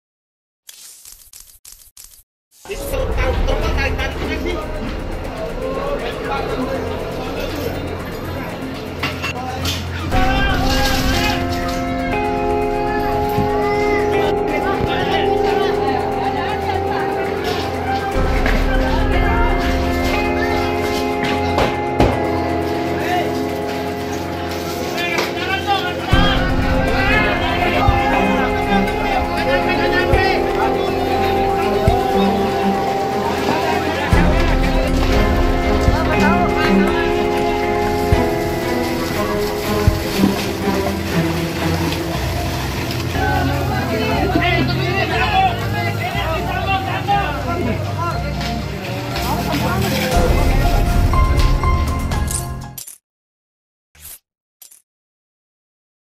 Video Kepanikan Warga saat Kebakaran Permukiman Padat di Kemayoran
Terjadi kebakaran di pemukiman warga daerah Kemayoran, Jakarta Pusat. Api diduga berasal dari korsleting listrik yang memicu ledakan. Sedikitnya tujuh rumah hangus terbakar.

Warga di sekitar lokasi kebakaran panik mencoba menyelamatkan barang berharga mereka. Kondisi bangunan yang semi permanen, membuat api menyebar dengan sangat cepat. Lokasi kebakaran yang berada di tengah pemukiman pun mempersulit usaha petugas pemadam kebakaran.